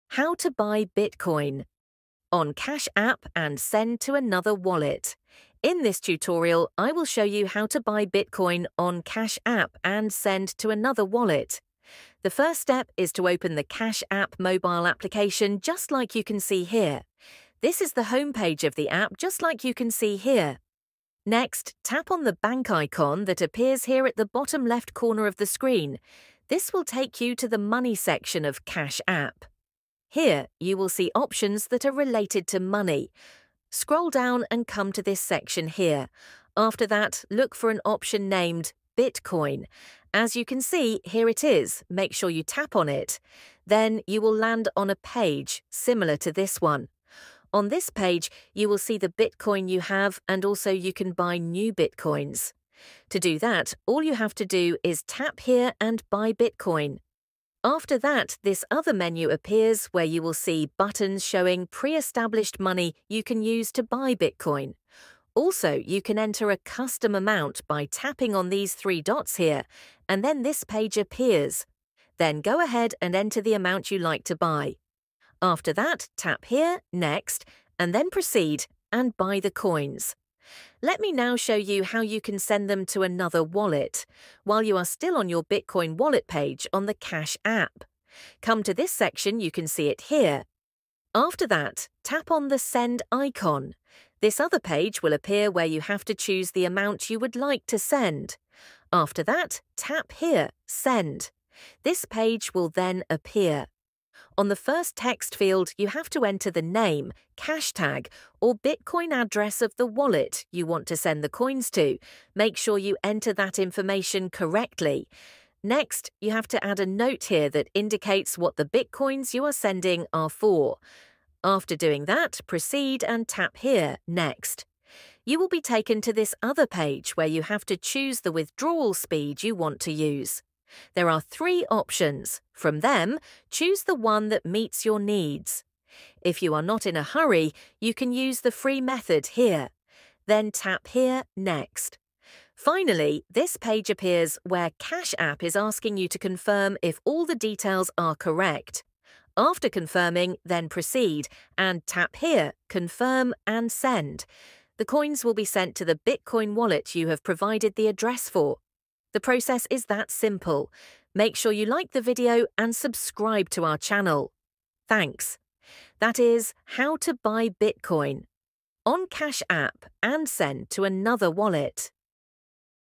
How To Buy Bitcoin On Cash App And Send To Another Wallet
watch this tutorial to see How To Buy Bitcoin On Cash App And Send To Another Wallet